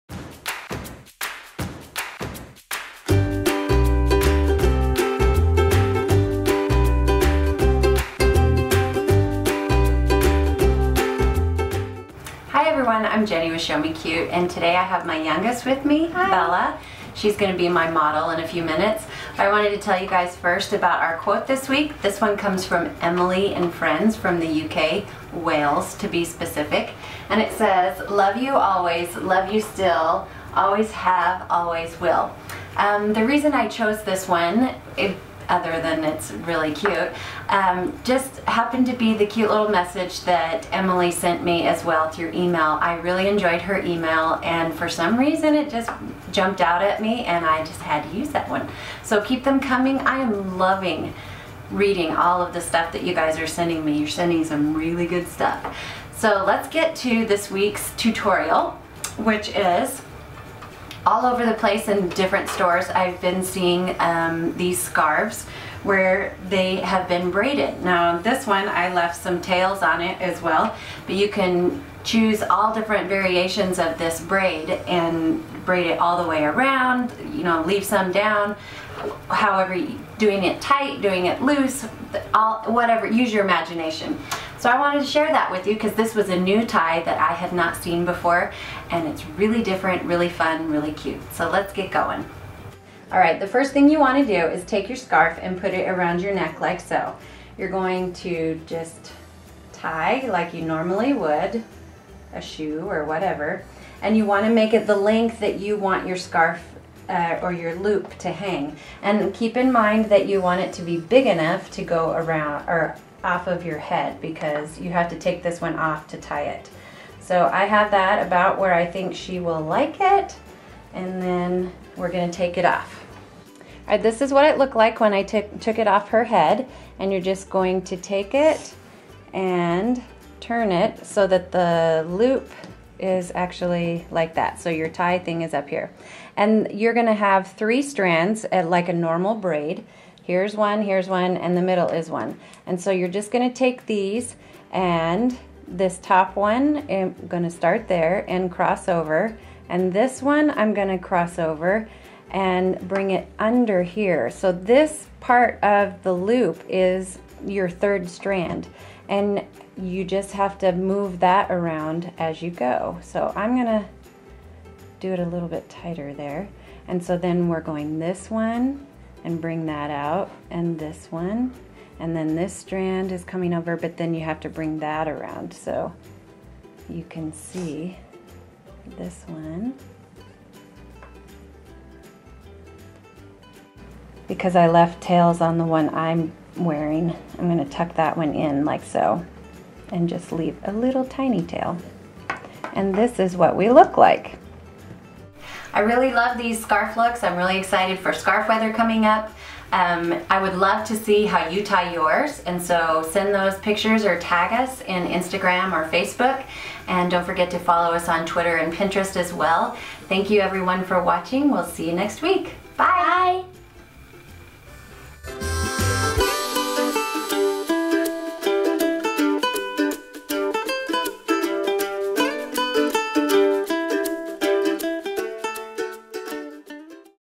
DIY: Braided Scarf | ShowMeCute
DIY: In this video I will show you how to use braiding techniques, usually used for hair, to braid a scarf. Bella, my youngest daughter helps me with this project.
Please keep sending your comments and phrases that mean something to you.
Love,
Jenny